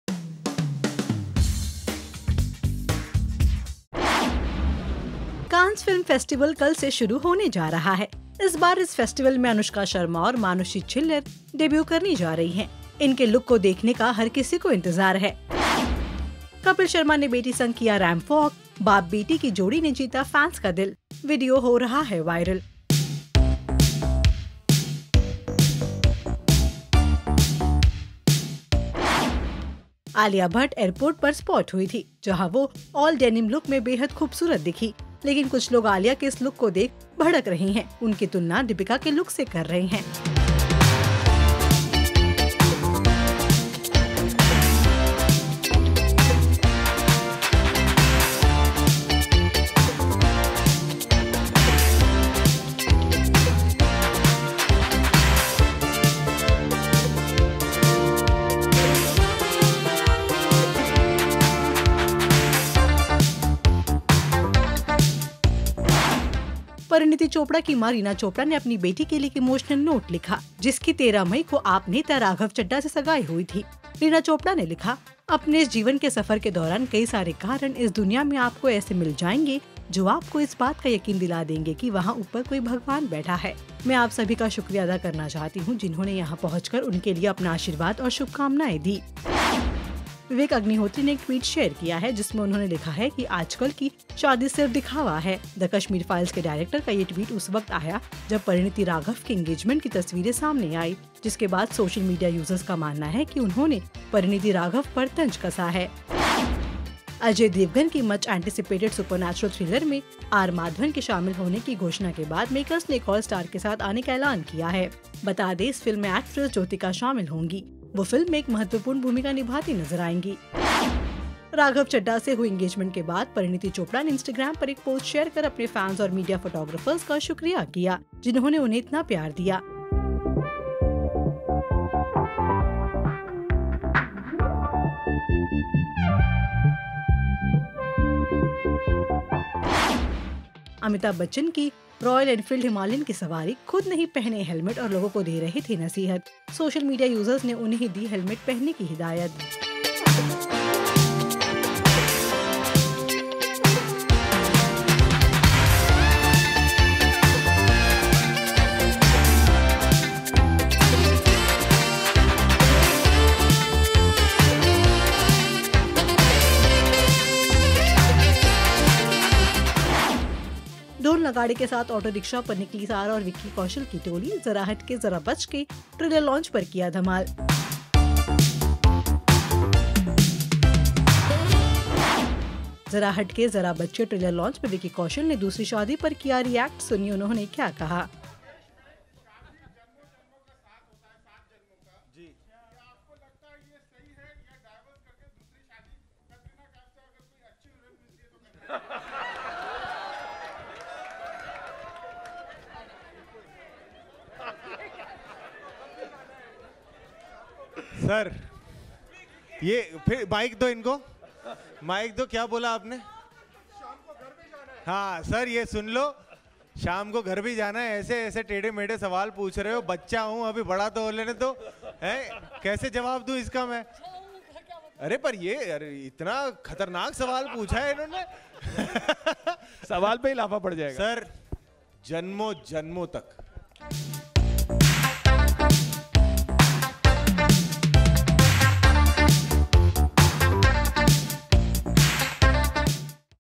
Cannes 2023, Kapil Sharma's Daughter's FIRST Ramp Walk, Alia Bhatt BRUTALLY Trolled | Top 10 News
Anushka Sharma and Manushi Chhillar's debut at Cannes 2023, Kapil Sharma's Daughter Anayra's FIRST ramp walk, Alia Bhatt BRUTALLY Trolled, Parineeti Chopra-Raghav Chadha engagement. Here are the Top 10 News of the Day in our Daily Wrap.